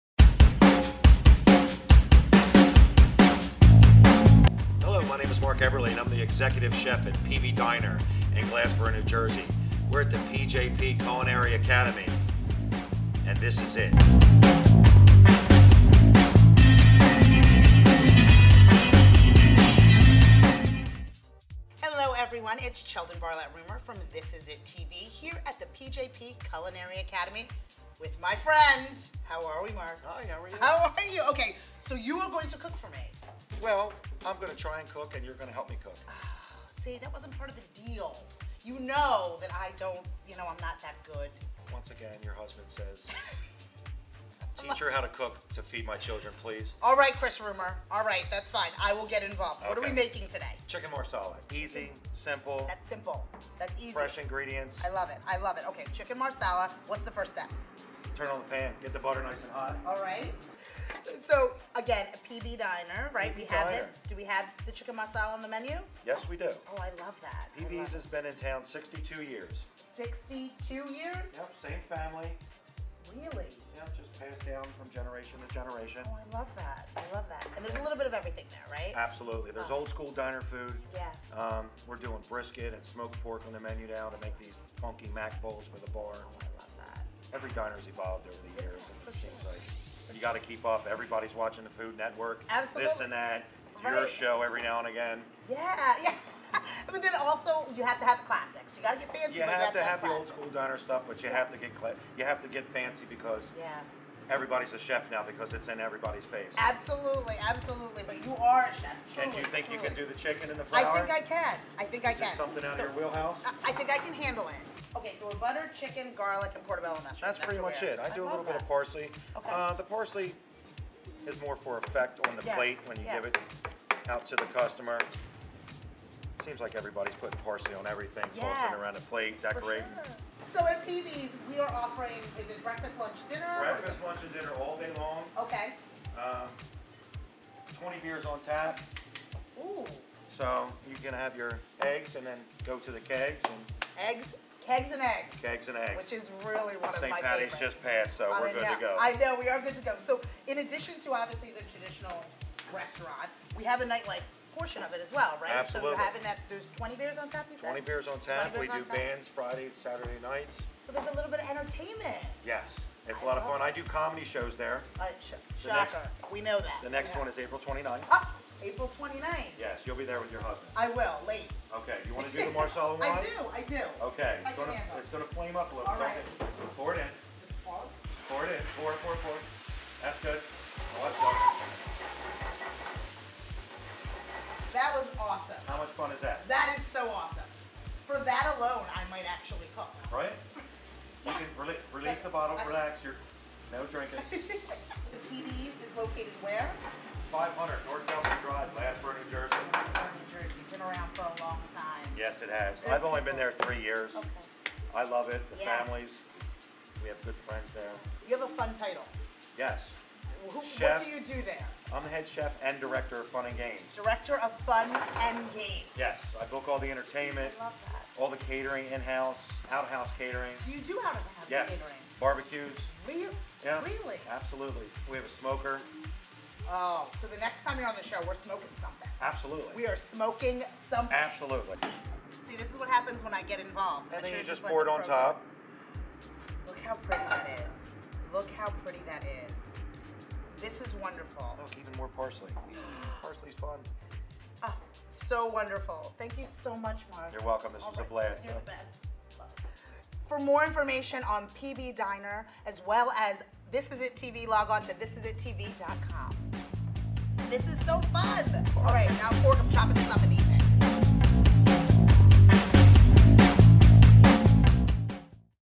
Chicken Marsala with Executive Chef Mark Eberle of PB's Diner
Learn how to cook Chicken Marsala with Executive Chef Mark Eberle of PB's Diner and Cheldin Barlatt Rumer from This is it TV!